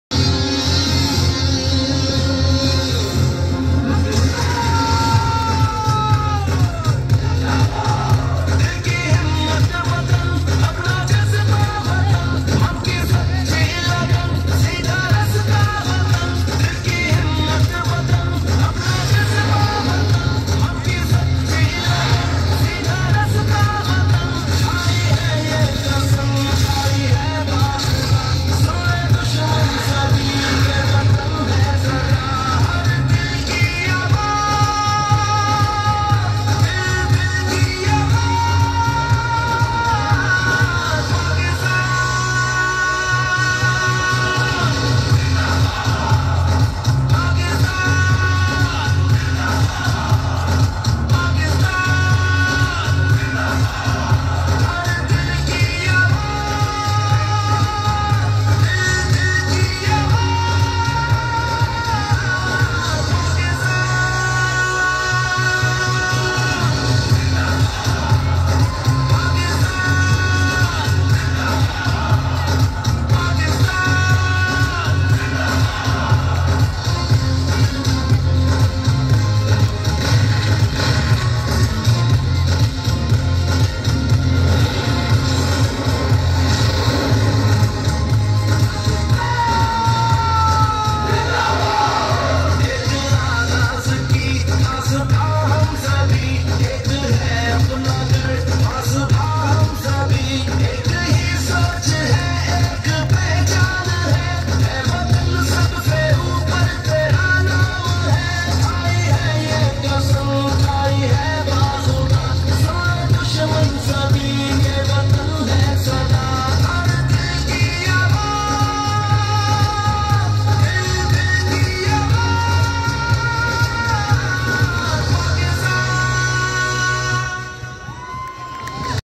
All Pakistan junior & senior cheerleading stunts championship 2022
All Pakistan junior & senior cheerleading stunts championship 2022 , on 26th March 2022, at NCC karachi